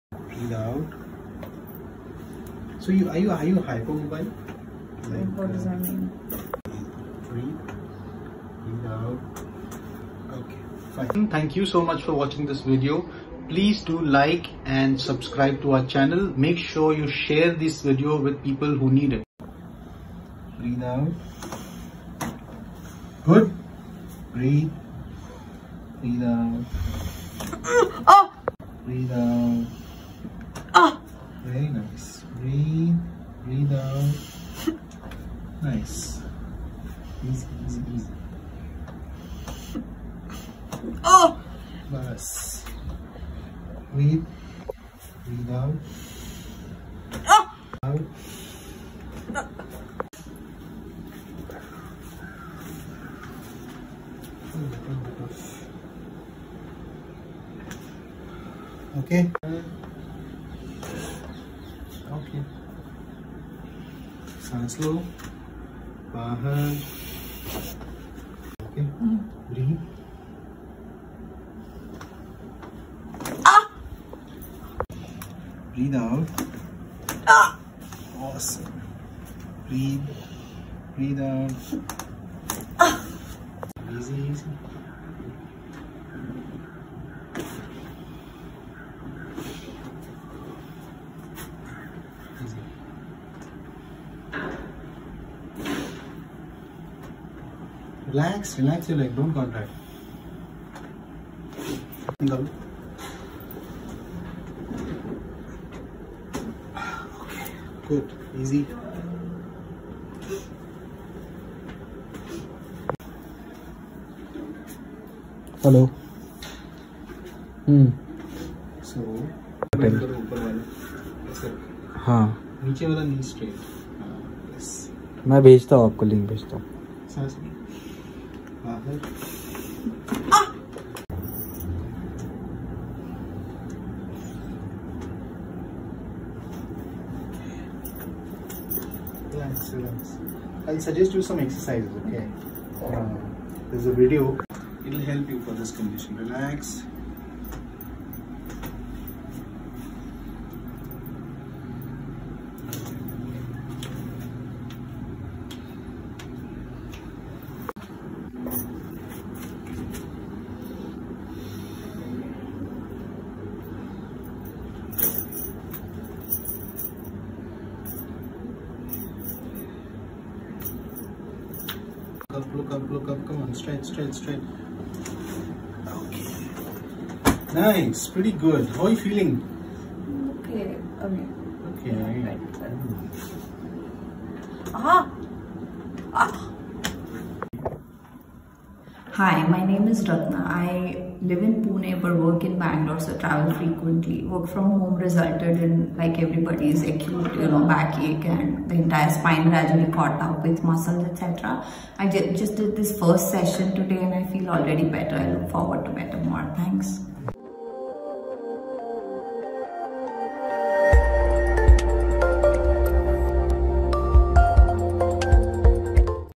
Acute back pain due to work from home relieved after a session of spinal manipulation
Ratna has been working from home and also been travelling for her work and this routine resulted in acute back pain and stiffness in back. We did a spinal manipulation session for her and she was able to feel the difference in her back and her pain had just vanished.

Check out what she has to say about her experience.